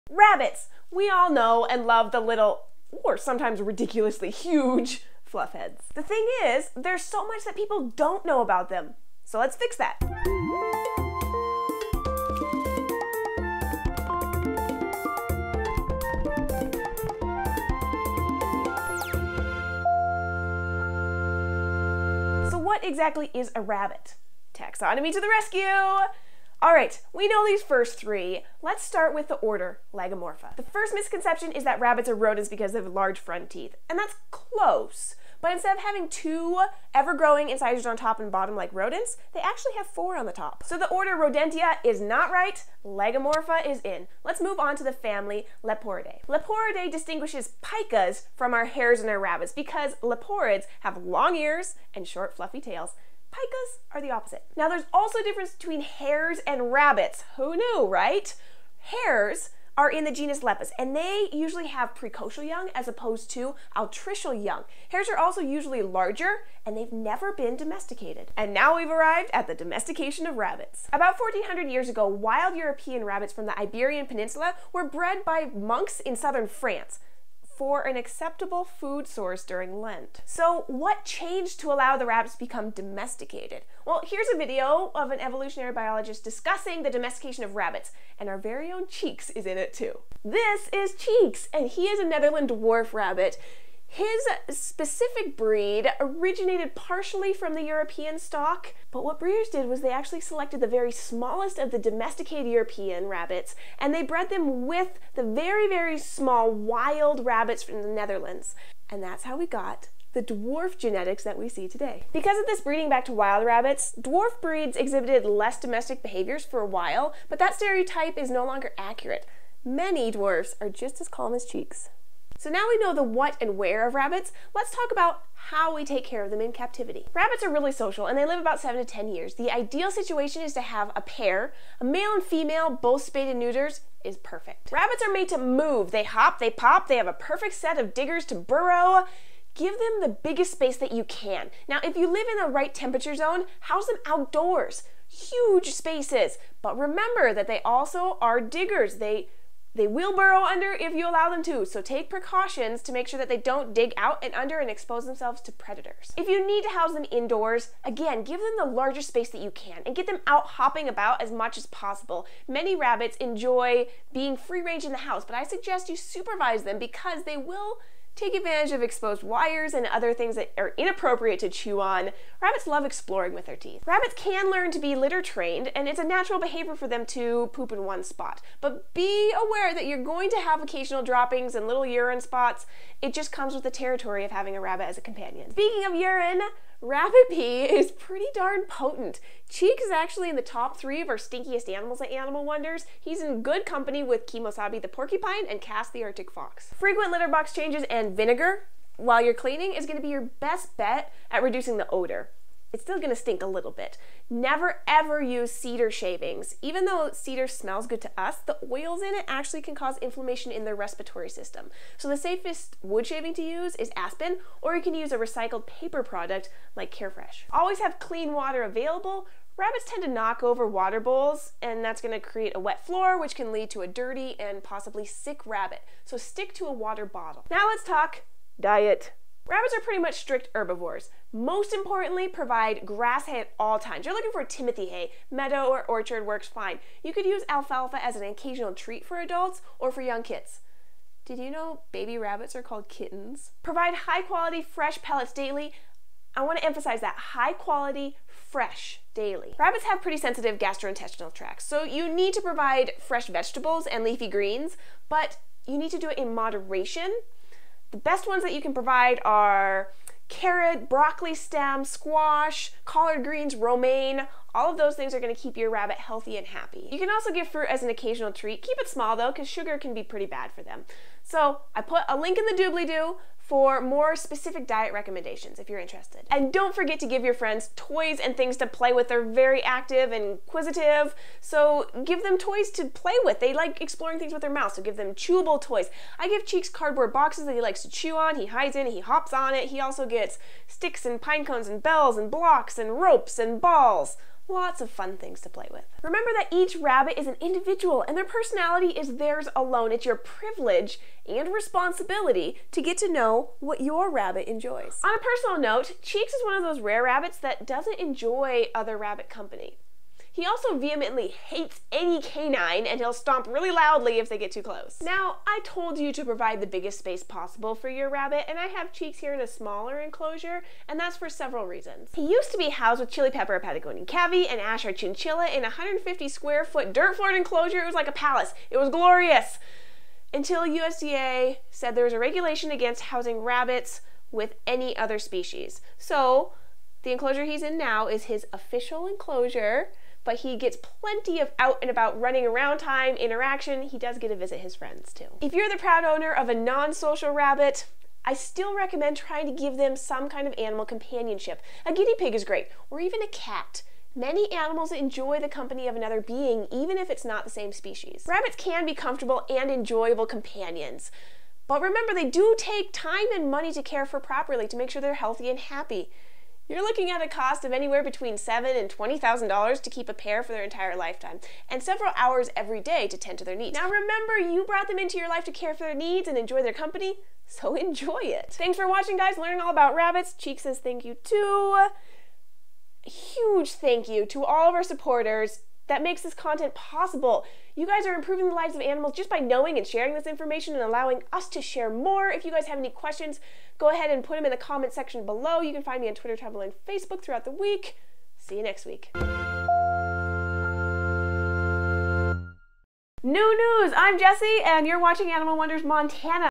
Rabbits! What, Where, and How
Jessi shares What rabbits are, Where they came from, and How to care for them in captivity. Cheeks plays the part of "rabbit".

Thanks to Ryan Hellegaard for his photos.
Thanks to Emily Pinner for her additional research.

Amazon Wishlist